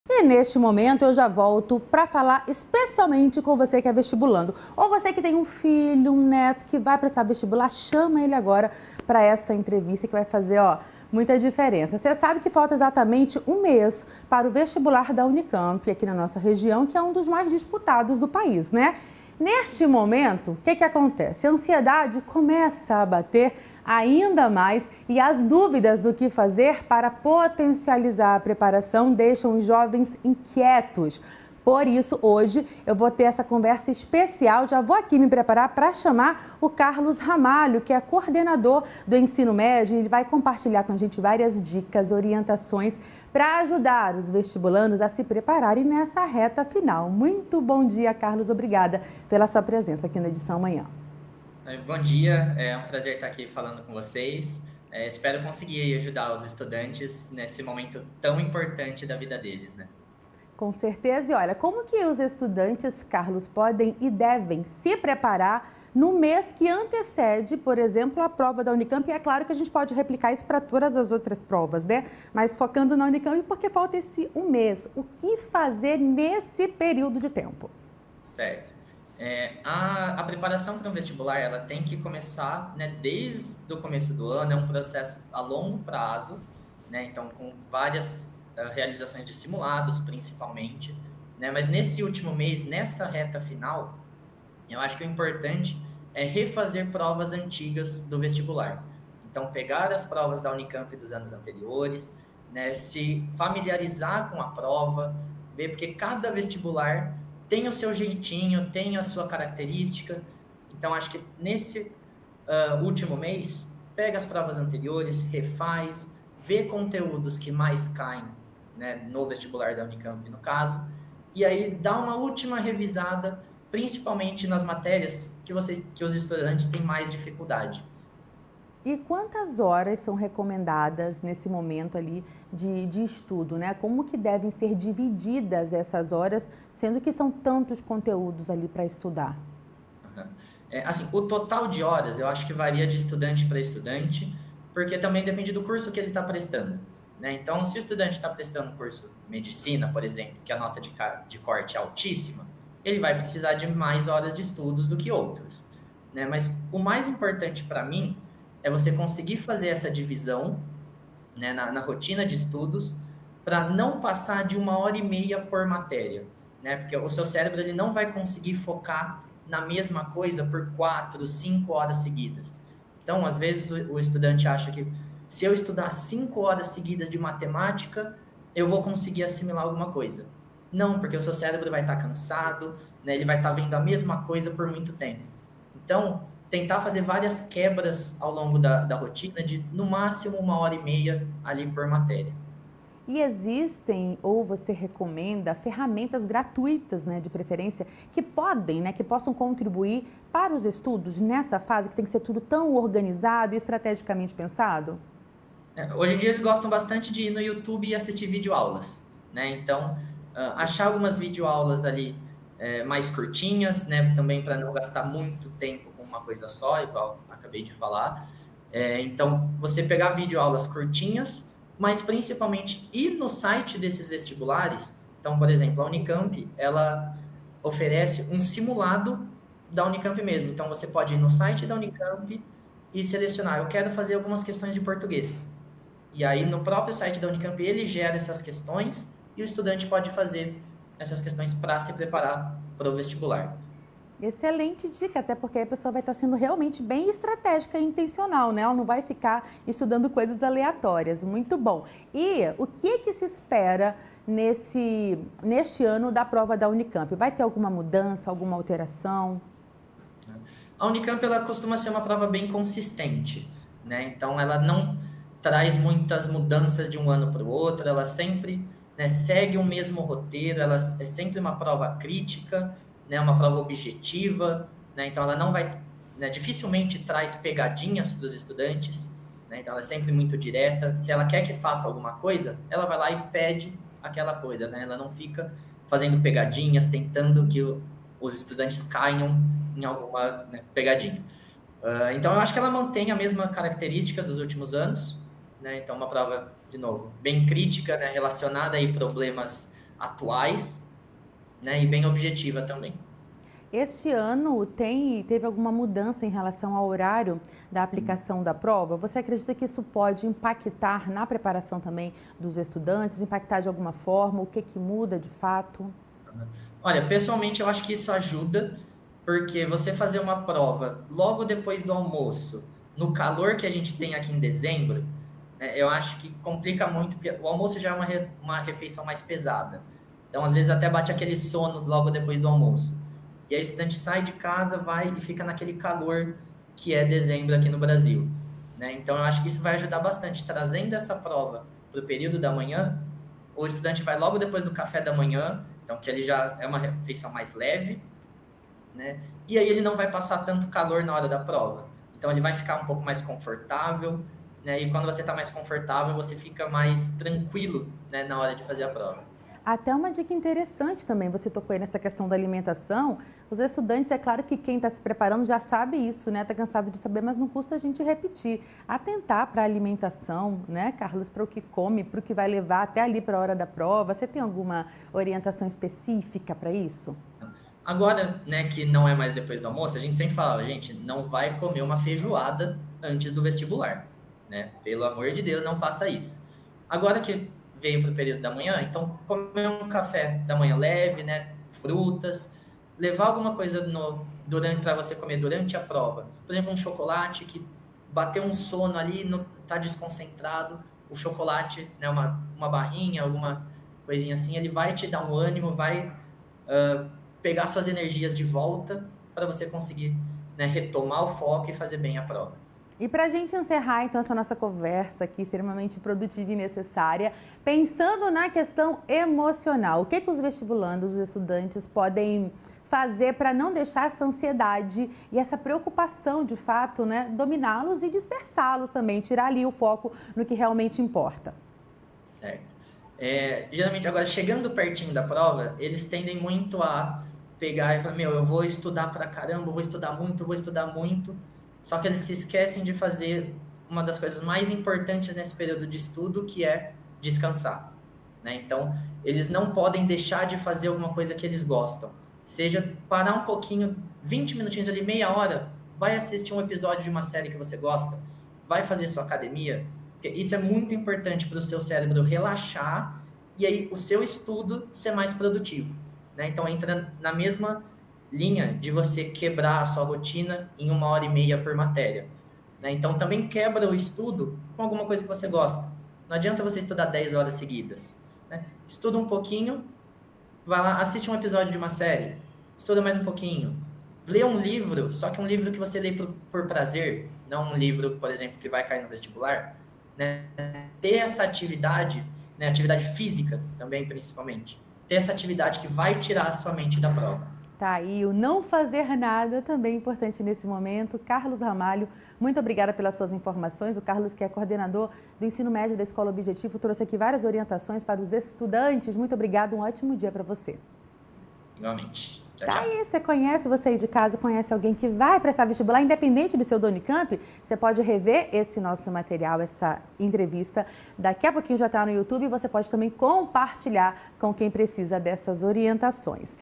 Edição Manhã - Vestibular chegando: Confira dicas para controlar a ansiedade nas provas
Carlos Ramalho, coordenador de Ensino Médio, compartilha dicas para a ajudar os vestibulandos a se prepararem durante a reta final para as provas.